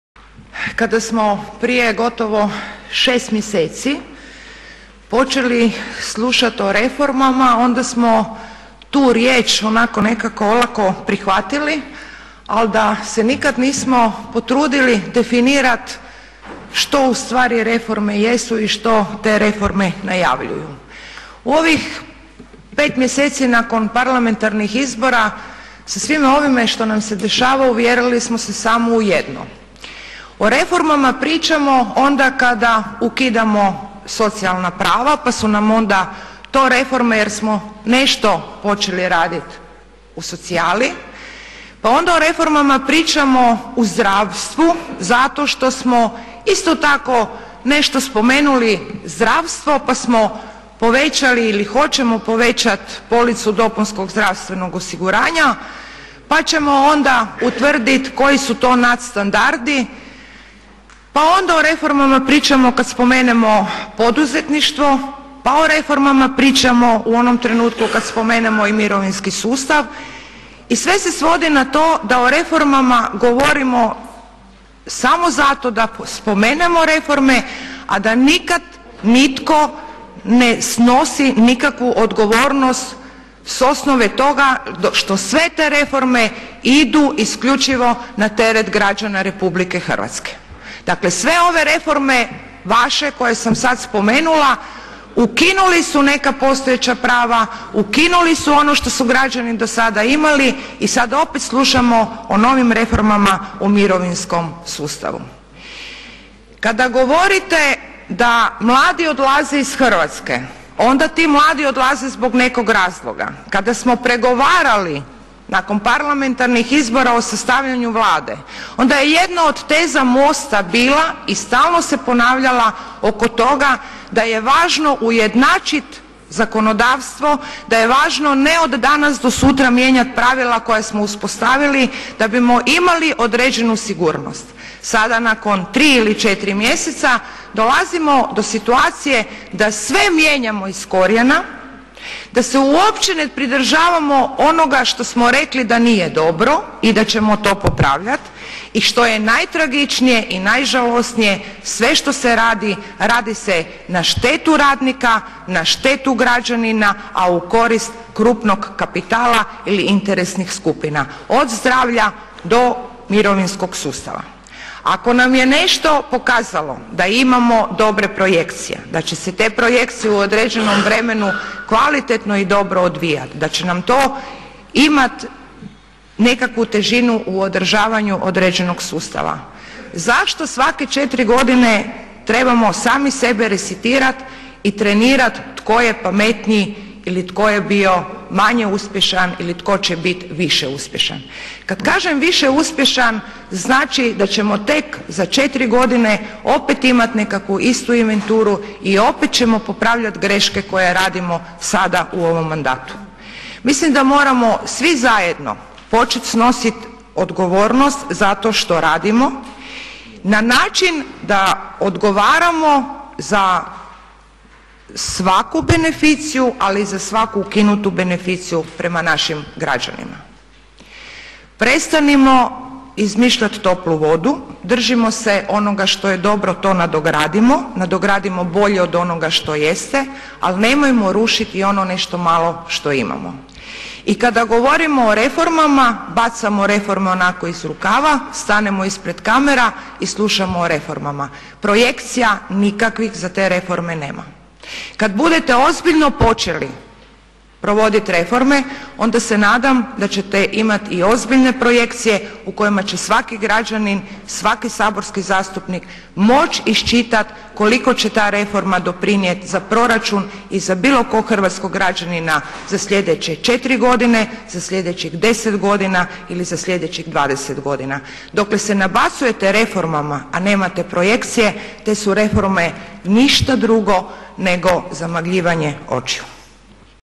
Laburisti | Reforme po Superhiku (Tireli)
Na najavu Vladinog programa reformi kojim je predviđeno brže izjednačavenje radnog vijeka za muškarce i žene, te će od 2023. godine svi raditi do 67. godine, kao i da će se u prijevremenu mirovinu moći s navršene 62 godine reagirala je Laburistica Nansi Tireli